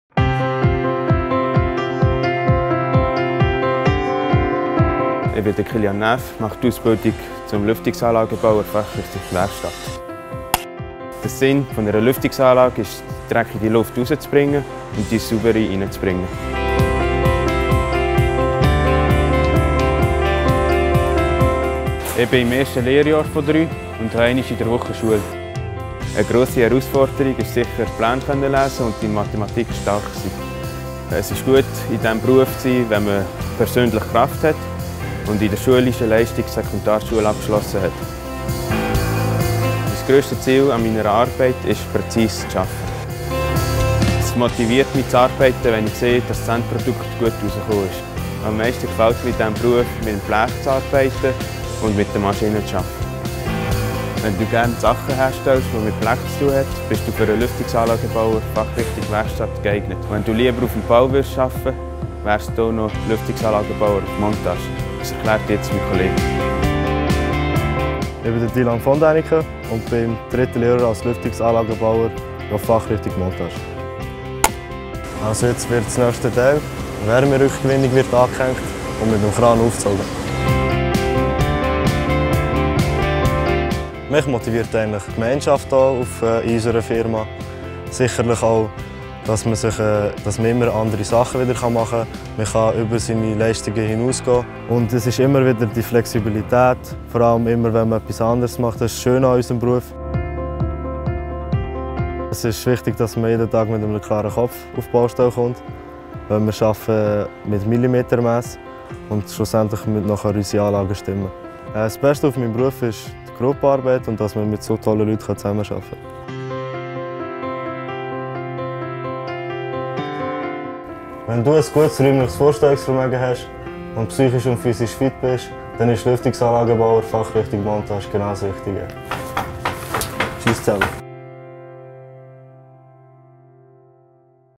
Lüftungsanlagenbauer/innen EFZ
Lüftungsanlagenbauer/innen EFZ sind Fachleute für die Herstellung und Montage von Lüftungs- und Klimaanlagen. Sie führen auch Wartungs- und Reparaturarbeiten aus.